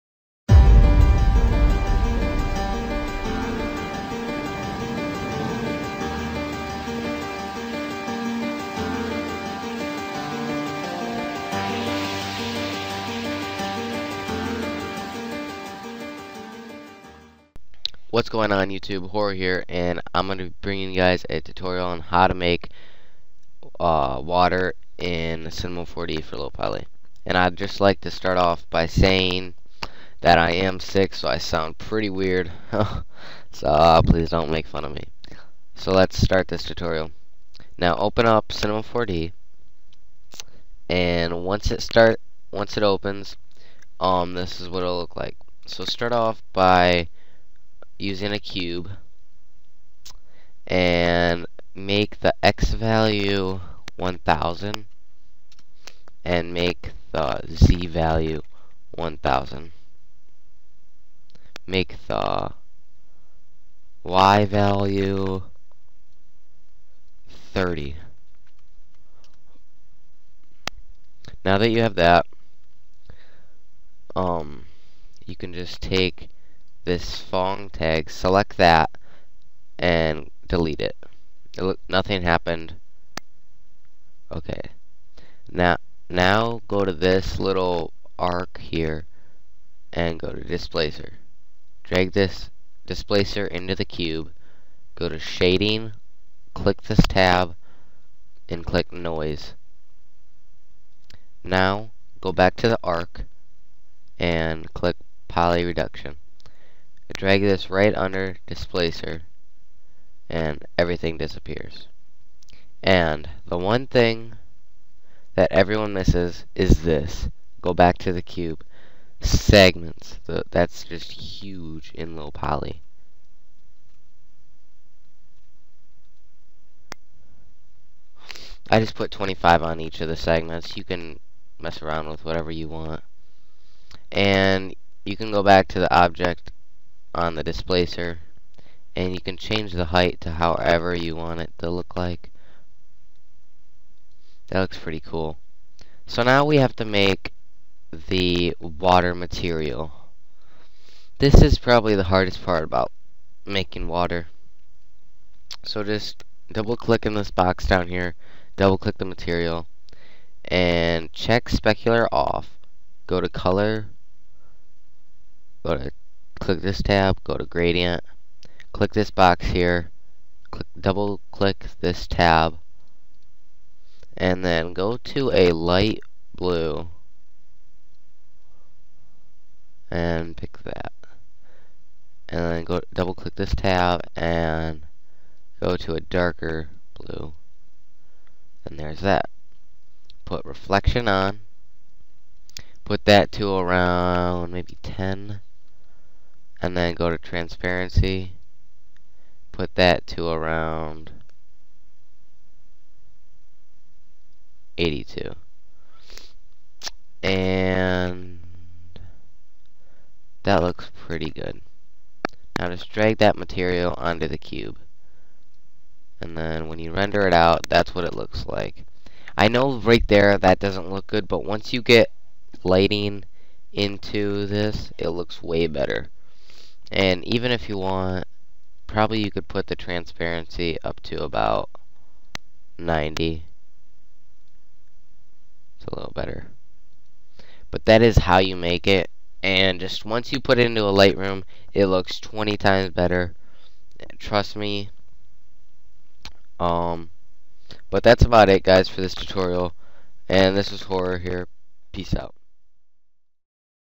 C4D Tutorial: Low-Poly Water - By Horror
Hey guys this tutorial is how to make water for low-poly. I hope it helps you guys out. This is going to be a series on my channel. I hope you all enjoy!

●Teams/Partners●